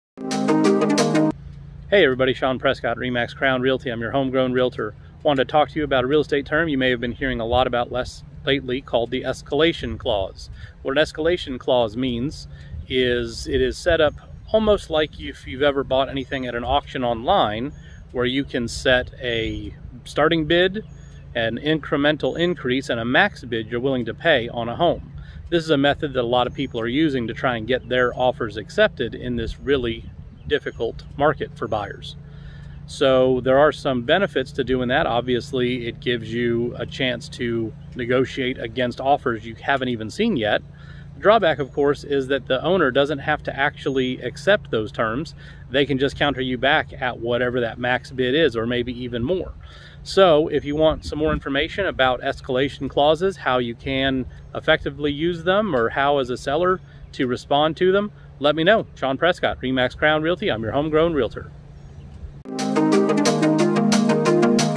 What is an Escalation Clause in Real Estate do and should I use one?
Quick explanation of an escalation clause on a real estate contract in Florida